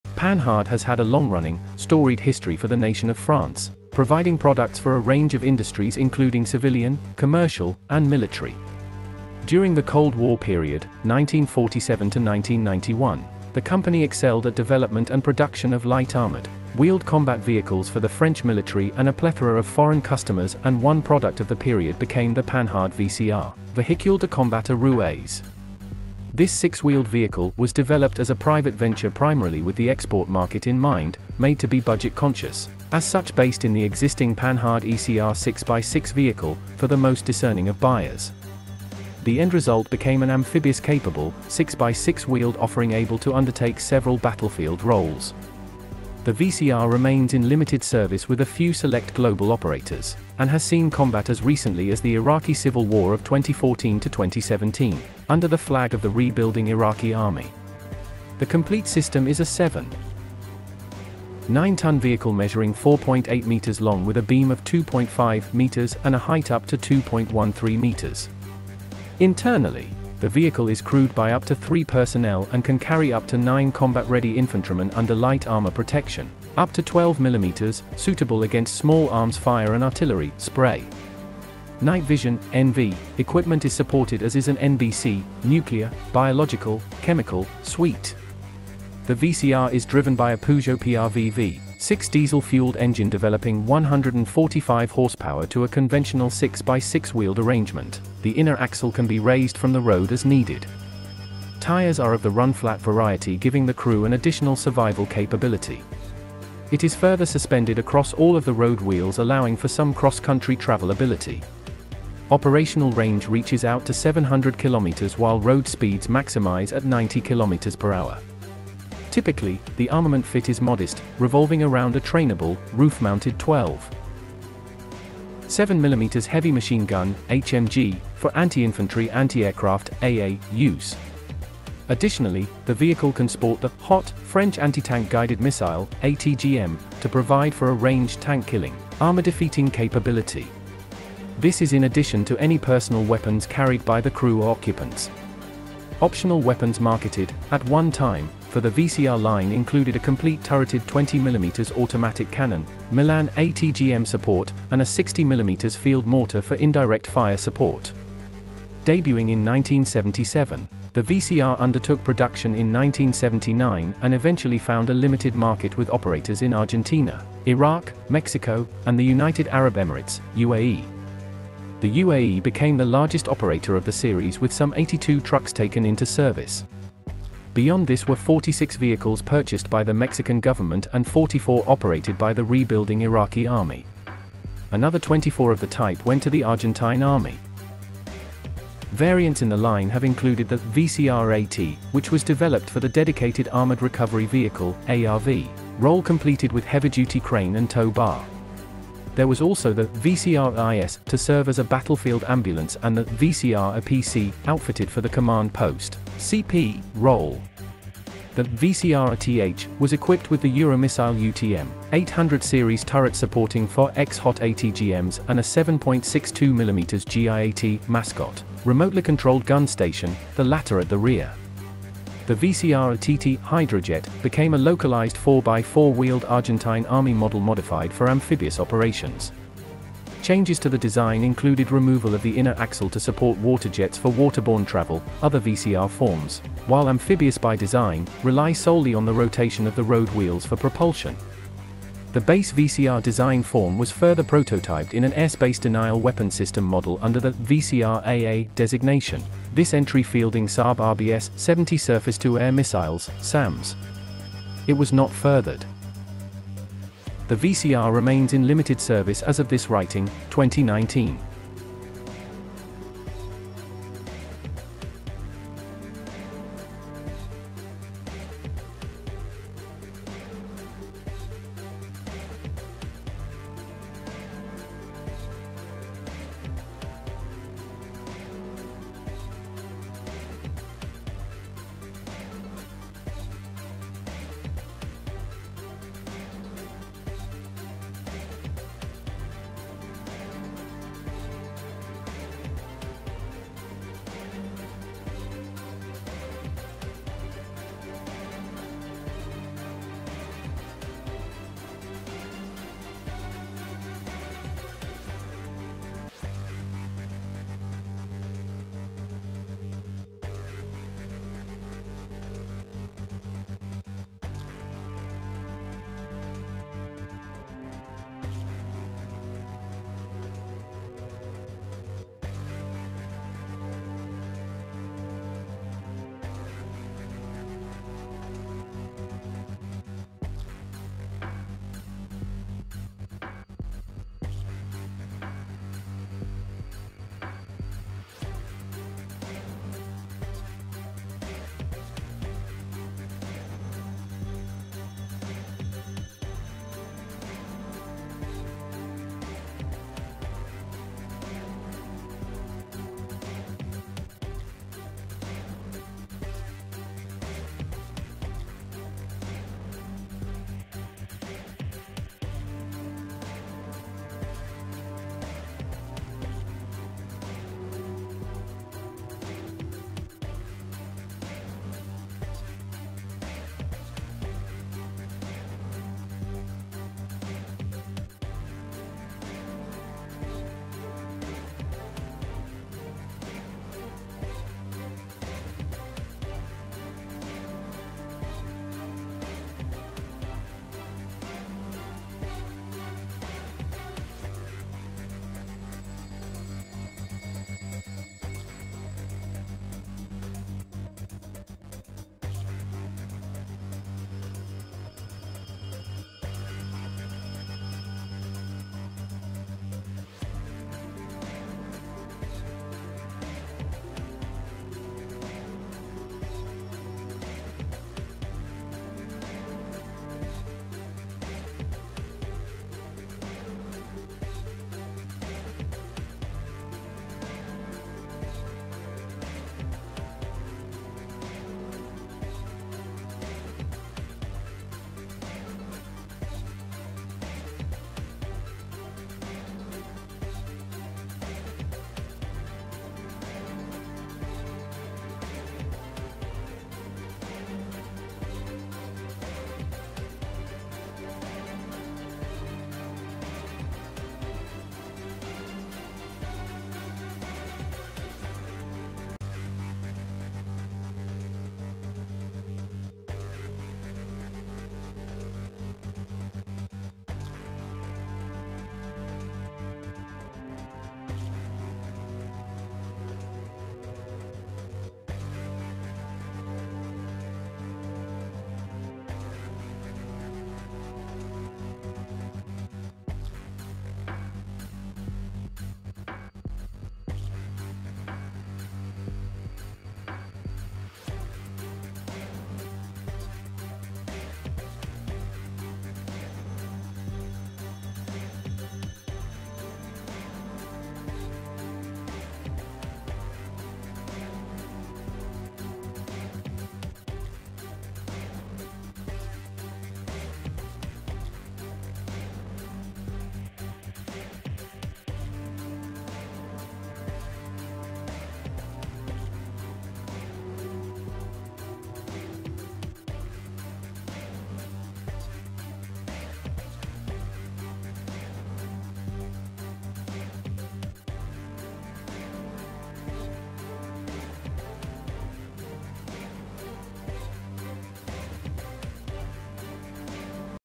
Panhard VCR
The French-made Panhard VCR Armored Personnel Carrier went on to see rather modest sales to foreign parties.